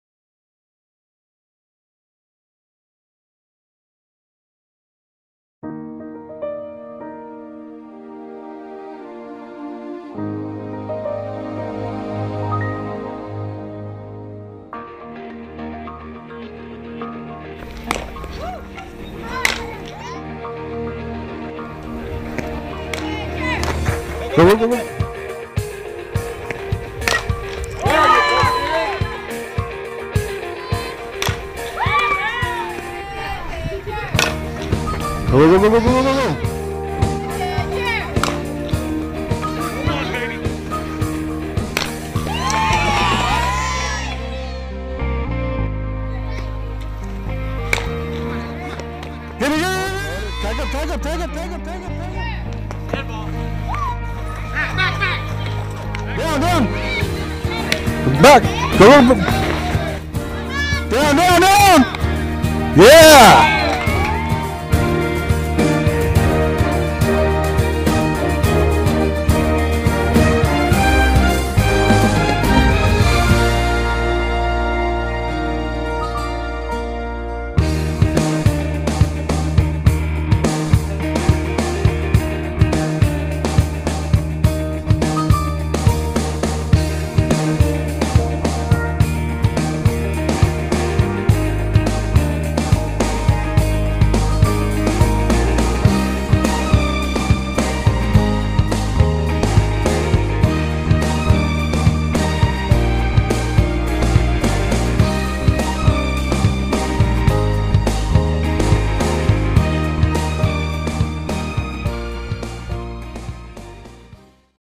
Canyon City Elite @ BLD Tournament vs 3 teams
A quick short clip of the Big League Dreams Tournament. Canyon City Elite came out swinging! Full highlights of each game coming soon!